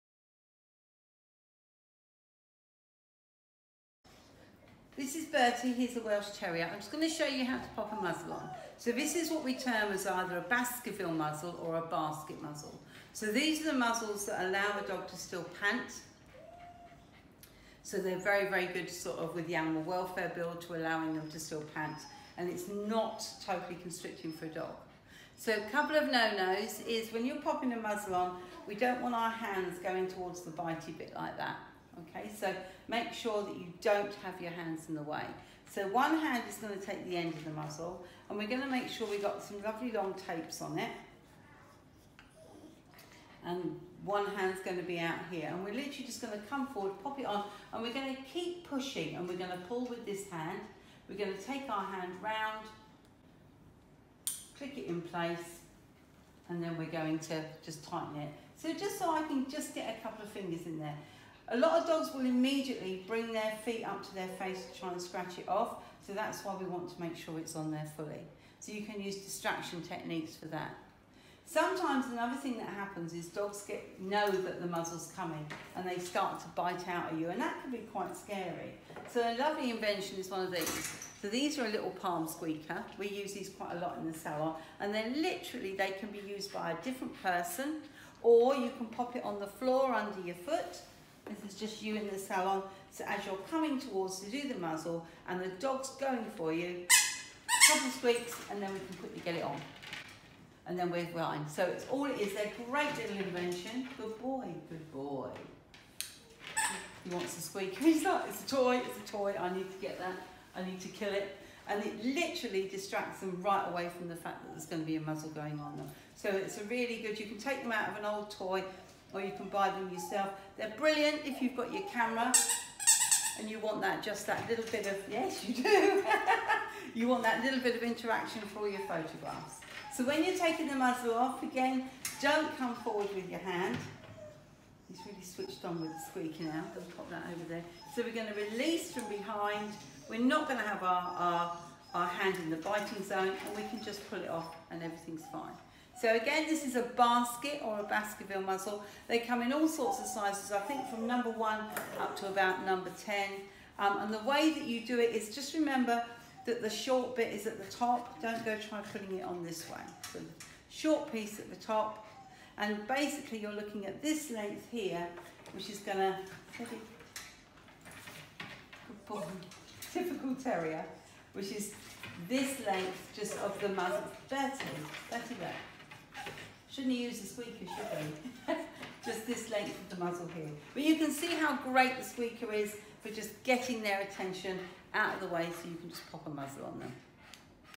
Muzzling a dog safely
Some dogs bite and so that you stay safe they need to wear a muzzle. Julie Harris shows how to safely put a muzzle on a dog using considerate techniques, including distraction toys.